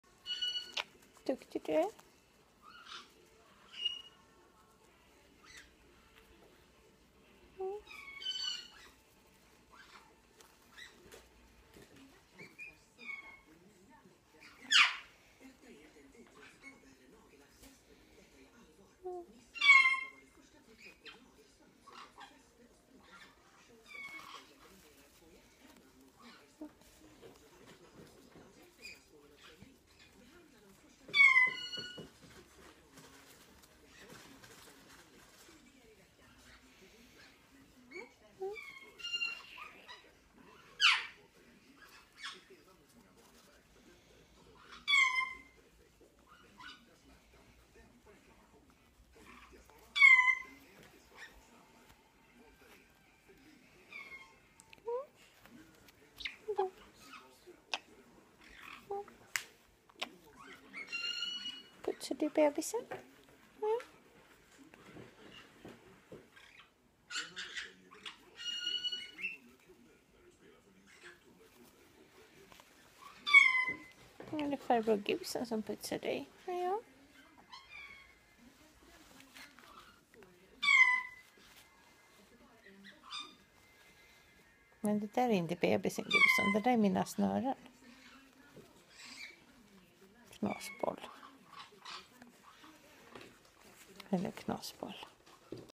Caique Gibson caring for a baby (not his but who cares :-)
Gibson cuddles and feeds not only his own babies, but as soon as opportunity arises he cares for the chicks in his neighbors nest! Such sweet daddy!!!
Svarthuvad Vitbukspapegoja